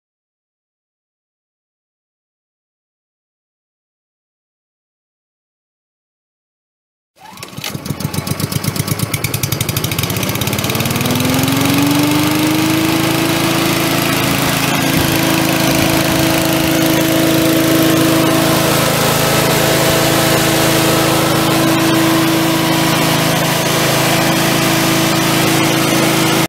Hurco sewer line testing equipment for sale | no-reserve Internet auction August 3, 2017
View a detailed listing of this item and place bids

Hurco sewer line testing equipment
Ripcord ventilator
Honda 5 HP gas engine
Serial RIP18H-10/24/01-022
18" to 30" manhole sealing plate
(2) 8" sewer plugs
(3) 10" sewer plugs
(2) 12" to 24" sewer plugs
(5) 30' poly lift lines with gauges
Part no. 261-408
It's located in House Springs, MO, and we're selling it to the highest bidder regardless of price.